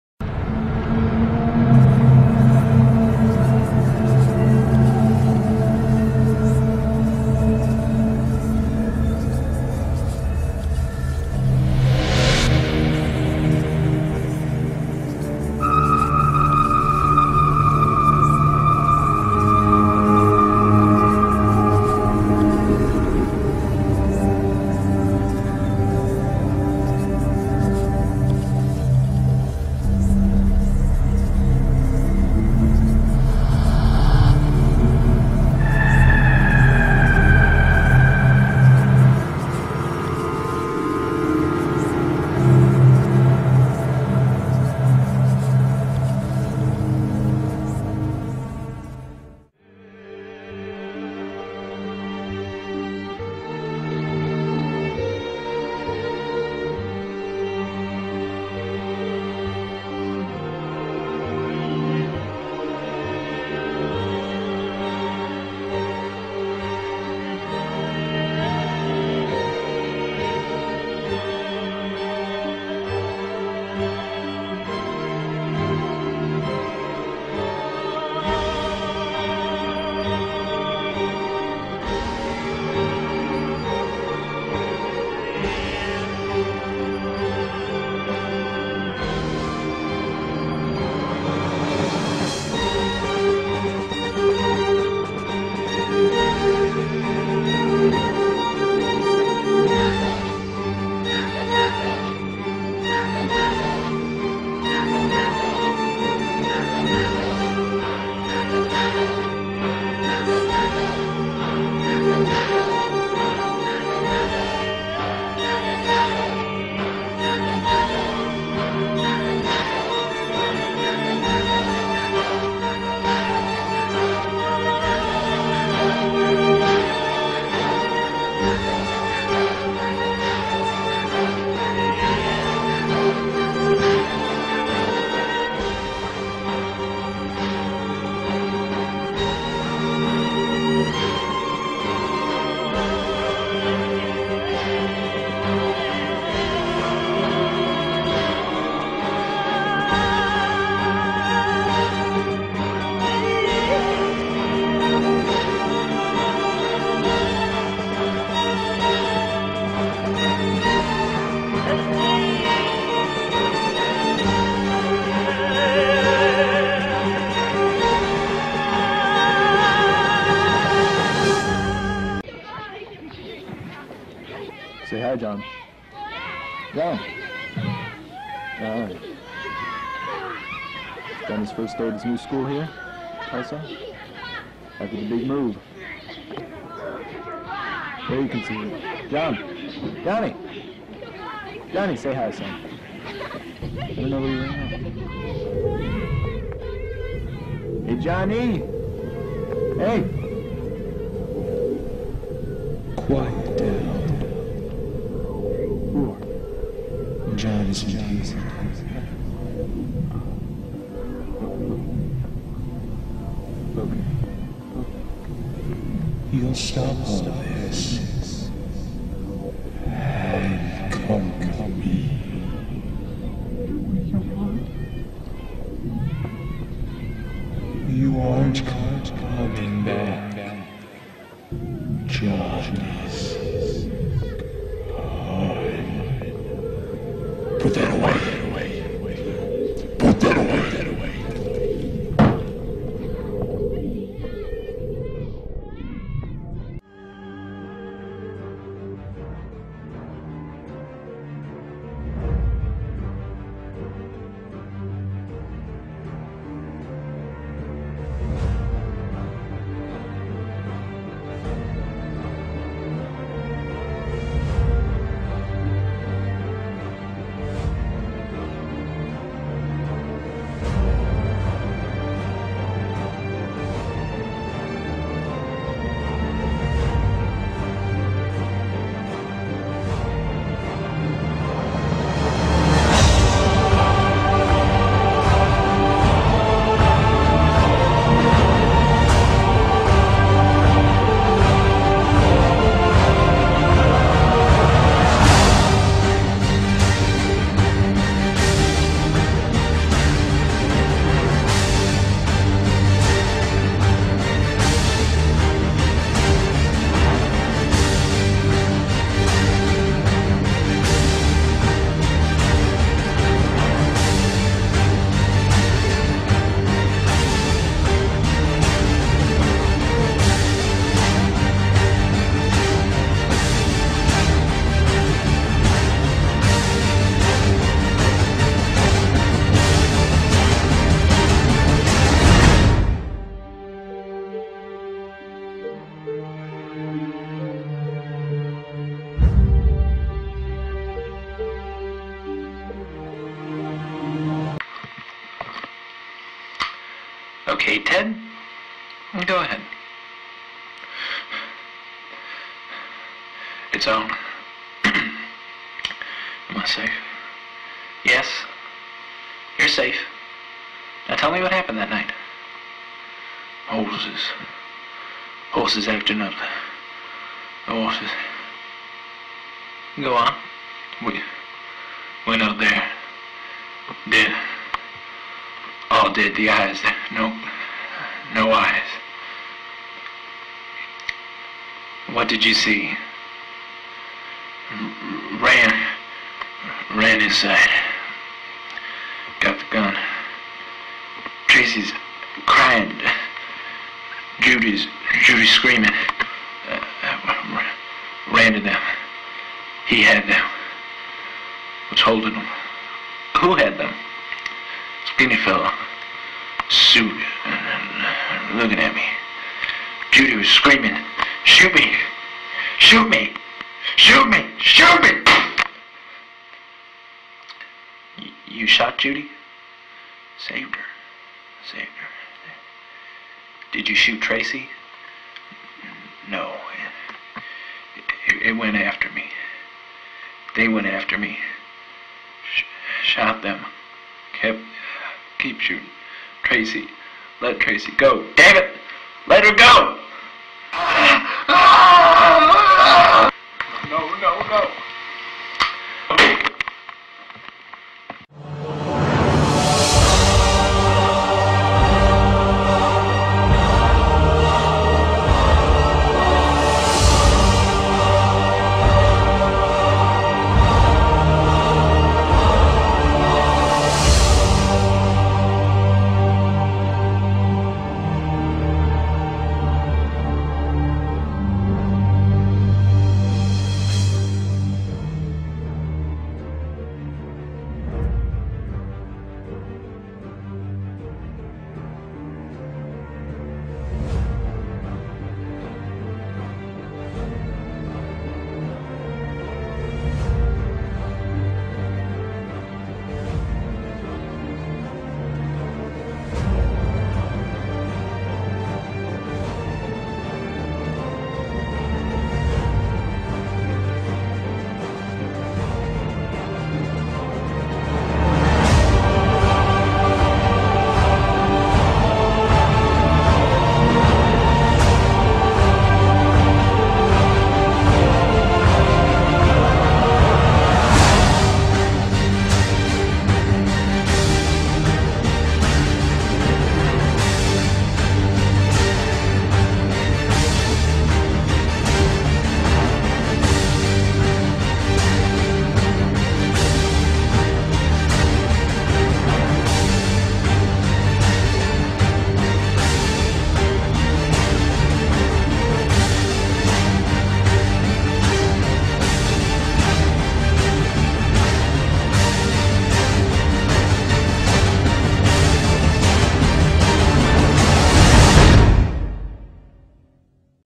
Slenderman Documentary
Scary and has slot of facts about Slenderman